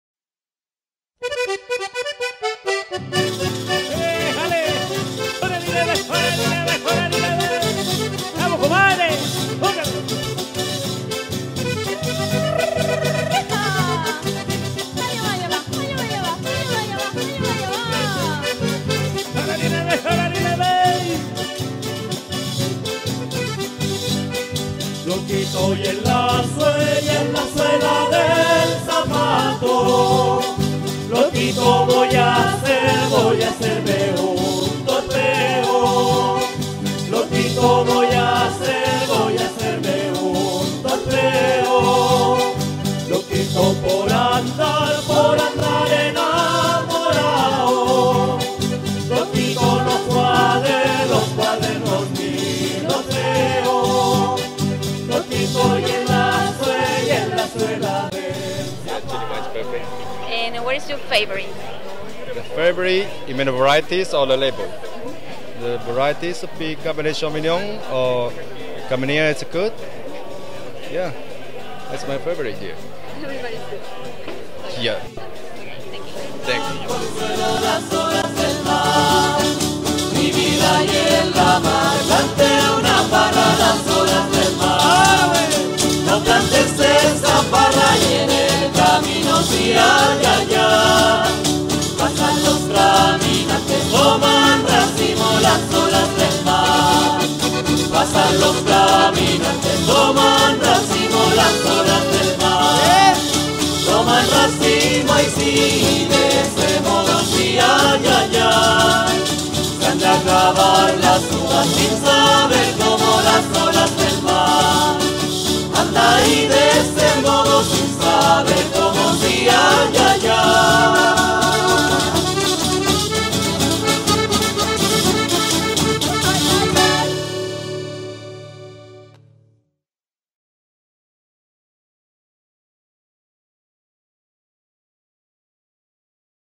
Fiesta de la Vendimia Santa Cruz 2013
Video resumen de la Fiesta de la Vendimia Santa Cruz 2013, edición final, que celebro con mas de 80.000 visitantes una de las fiestas mas importantes de nuestra region.